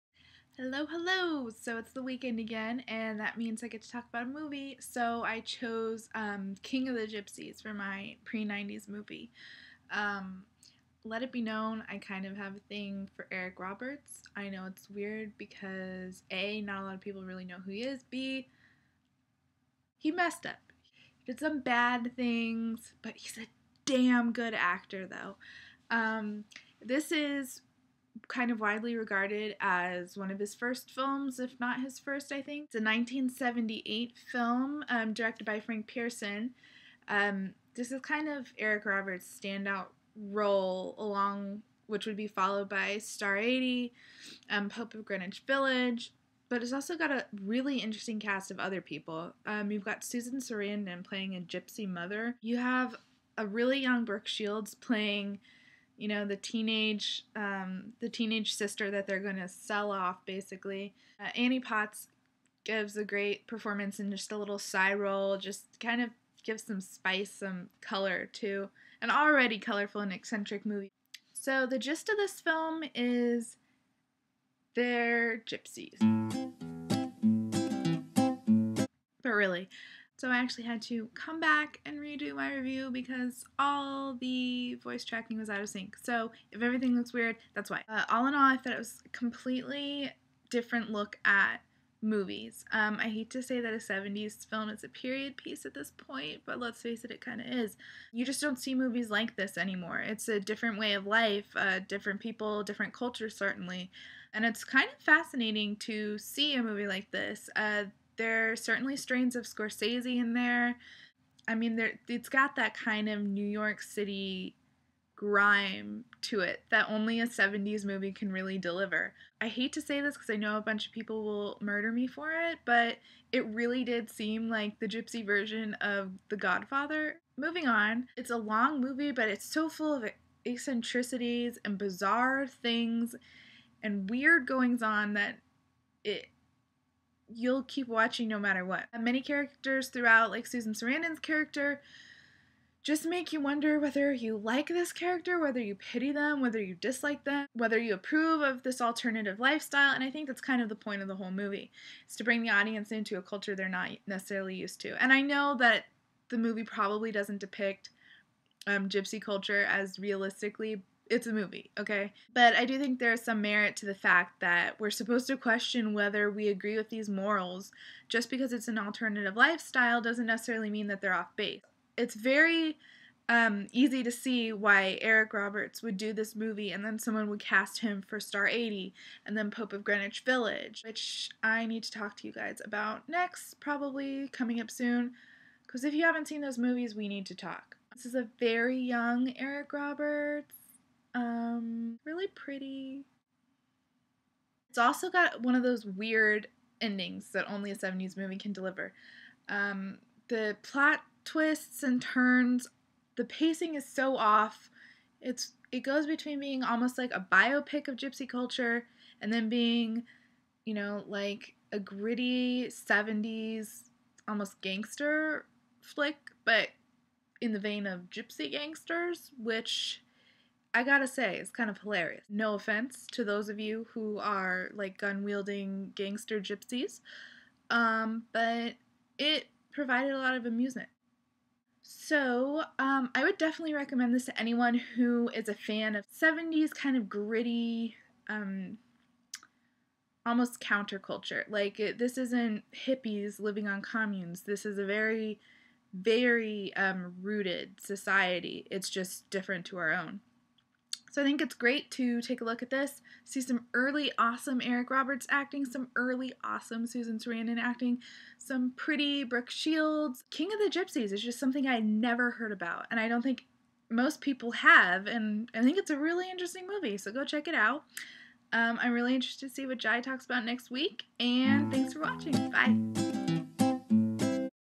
'King of the Gypsies' (Frank Pierson, 1978) - Review
My review of the film 'King of the Gypsies,' and a good deal of myself fawning over Eric Roberts (the superior Roberts sibling).